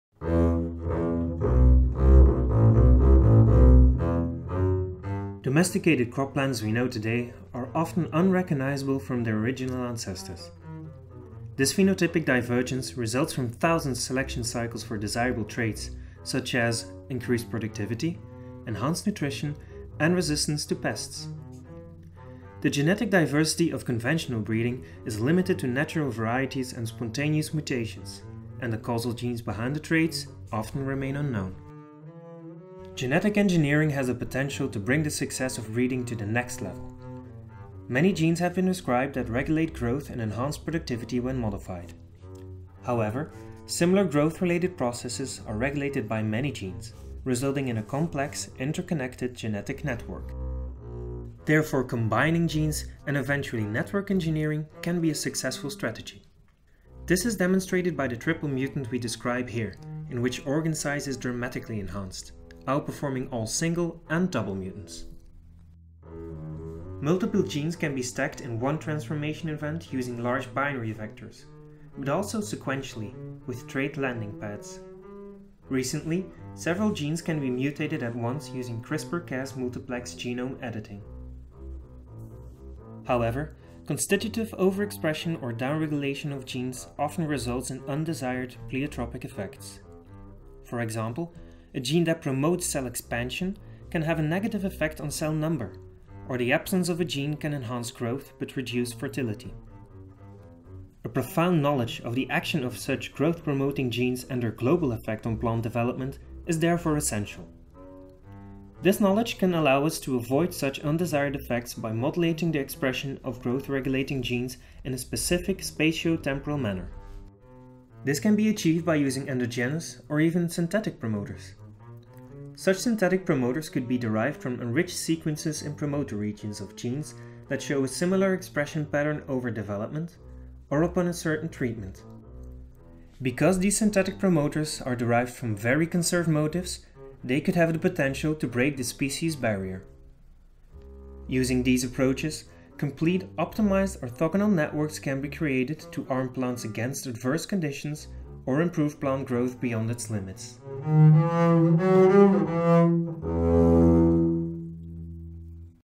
PLANTS Plant growth beyond limits
Produced by Hannes Vanhaeren, Dirk Inzé, and Nathalie Gonzalez (Department of Plant Systems Biology, VIB, Gent, Belgium). Read the related Opinion article in the February 2016 issue of Trends in Plant Science